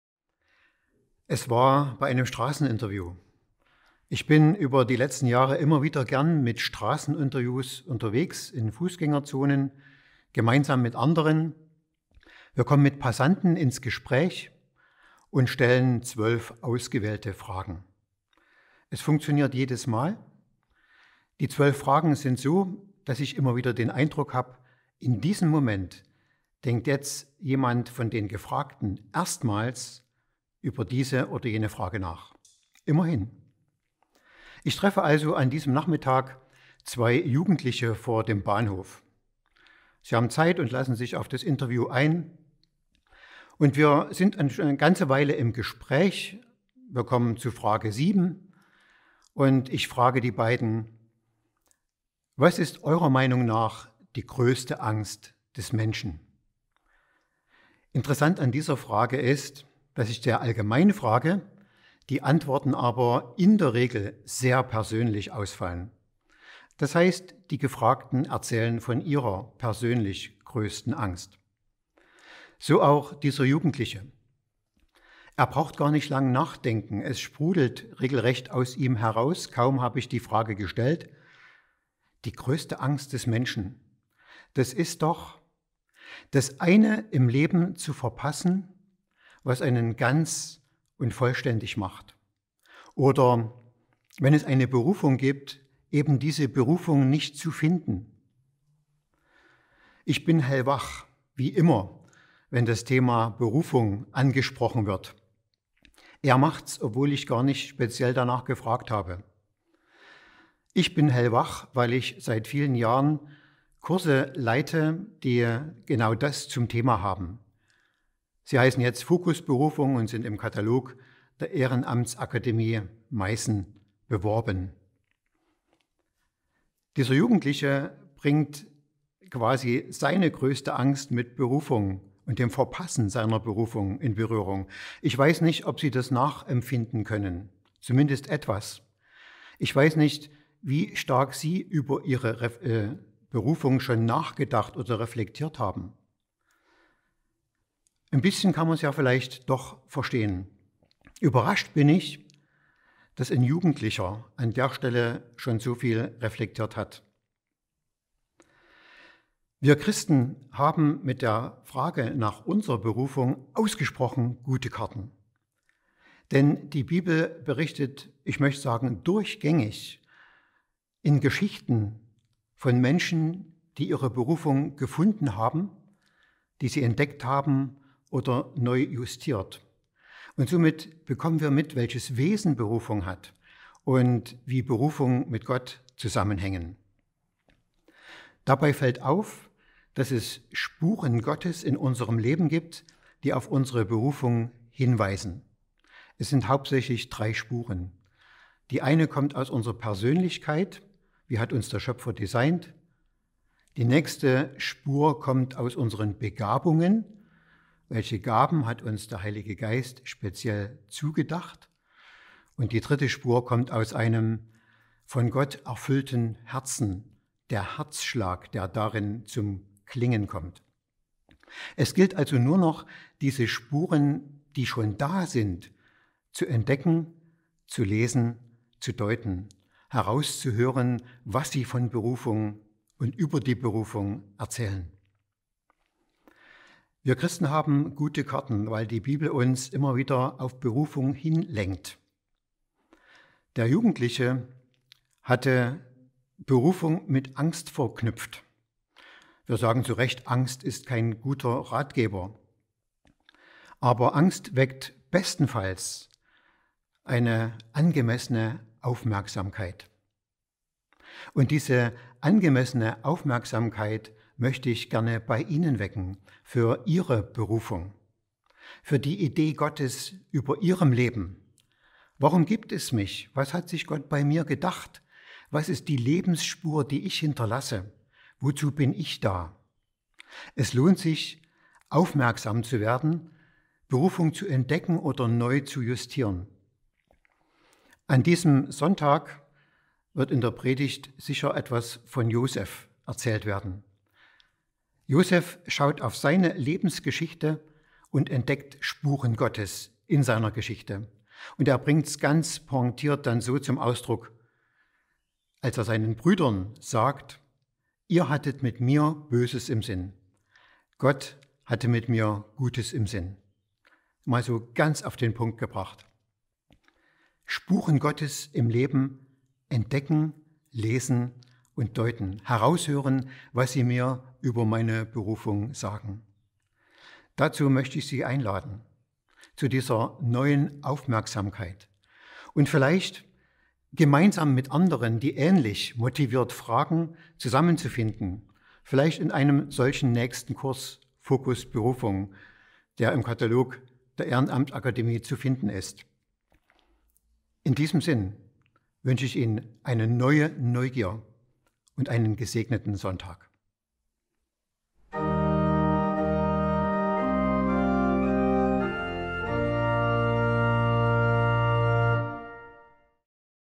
Laurentius online | 27.6.2021 | 4. Sonntag nach Trinitatis
Was ist meine Berufung?
Prädikant Friedemann Schwinger lädt ein, neu darüber nachzudenken:
Was ist meine ganz persönliche und konkrete Aufgabe in dieser Welt?
Welche Spuren Gottes kann ich in meinem Leben entdecken, die mir helfen meine Aufgabe zu finden?

Ev.-Luth. Laurentiuskirchgemeinde
IBAN: DE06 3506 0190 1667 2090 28
Spendenzweck: RT 1002 Onlinekollekte